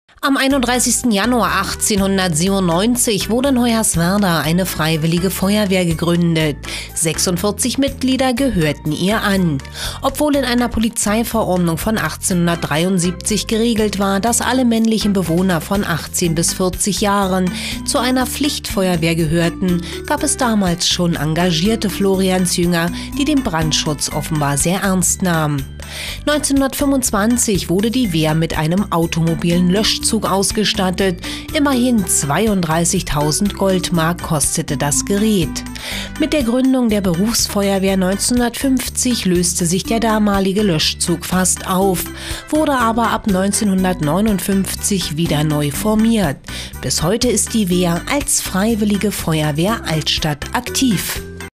Freiwillige Feuerwehr Hoyerswerda gegründet
Am 31. Januar 1897 wurde in Hoyerswerda eine Freiwillige Feuerwehr gegründet. 46 Mitglieder gehörten ihr an. Obwohl in einer Polizeiverordnung von 1873 geregelt war, das alle männlichen Bewohner von 18 bis 40 Jahren zu einer Pflichtfeuerwehr gehörten, gab es schon damals engagierte Floriansjünger, die den Brandschutz offenbar sehr ernst nahmen. 1925 wurde die Wehr mit automobilen Löschzug ausgestattet, immerhin 32.000 Goldmark kostete das Gerät. Mit Gründung der Berufsfeuerwehr 1950, löste sich der damalige Löschzug fast auf, wurde aber ab 1959 wieder neu formiert. Bis heute ist die Wehr - als Freiwillige Feuerwehr Altstadt - aktiv.

(Festschrift 100 Jahre Freiwillige Feuerwehr Hoyerswerda)